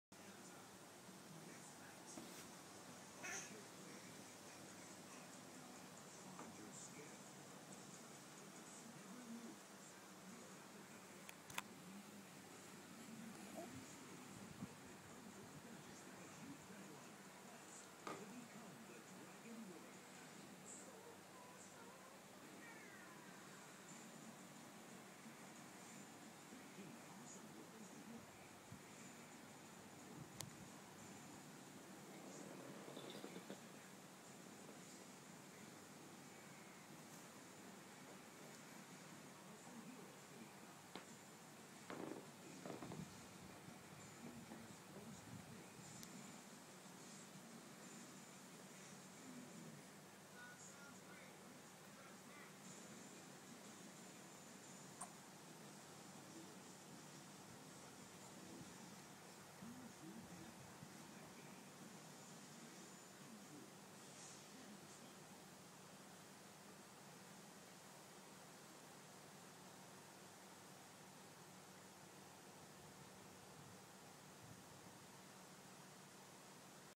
Tovi practice standing
Practice makes perfect. Exploring everything with his mouth is Tovi's latest hobby.